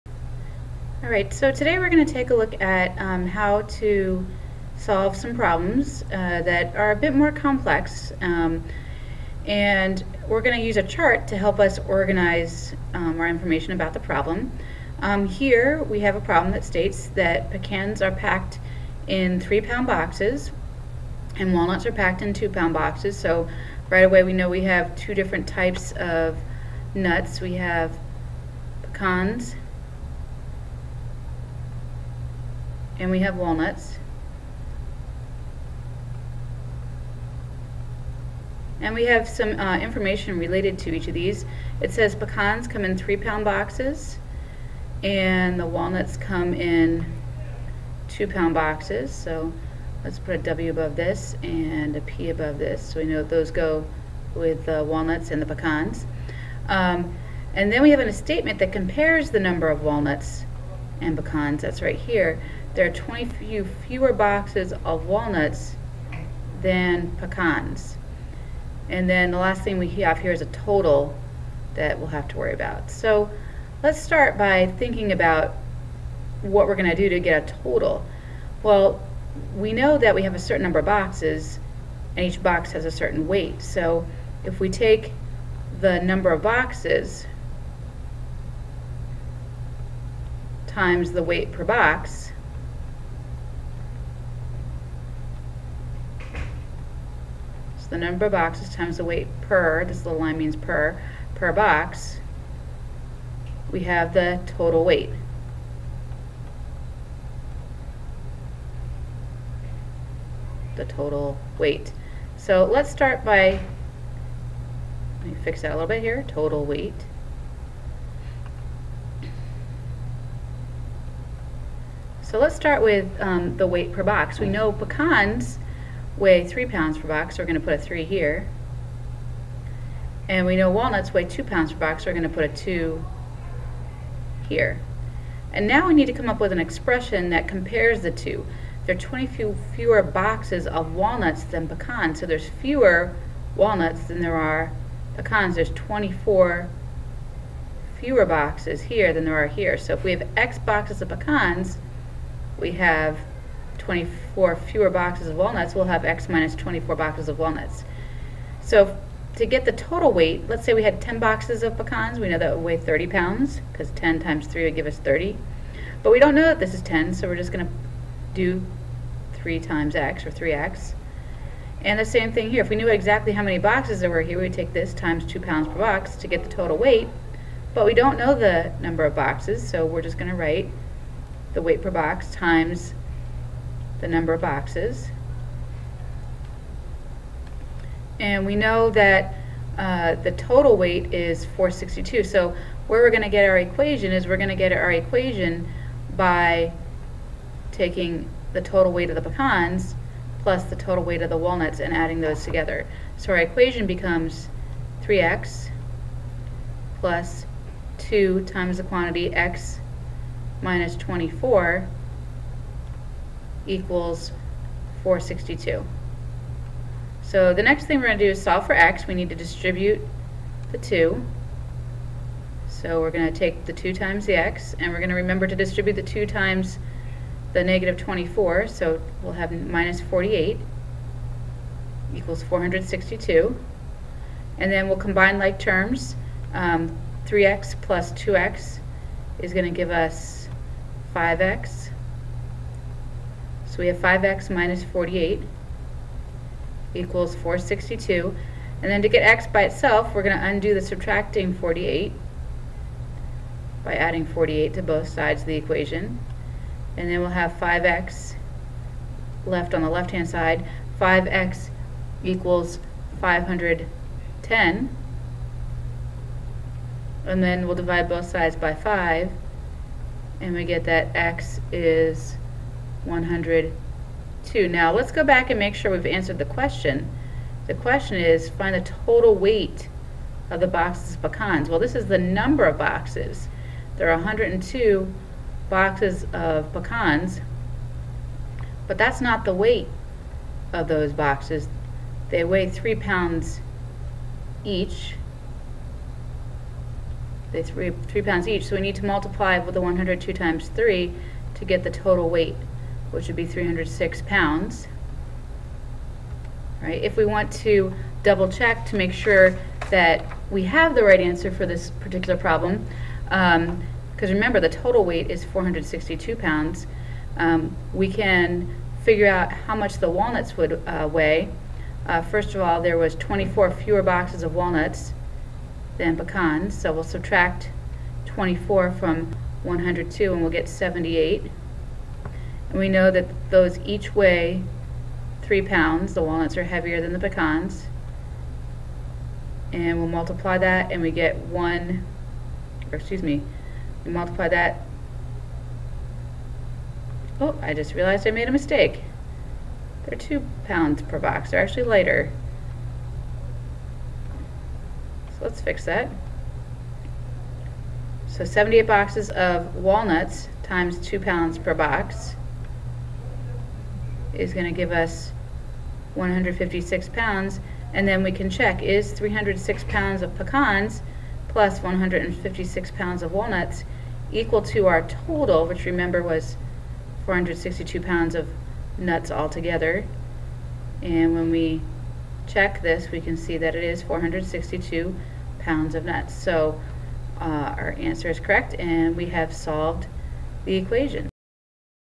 Algebra 1 Lesson 3.6 Problem Solving: Using Charts
This screencast shows an example of how to create a chart to solve an algebra application problem.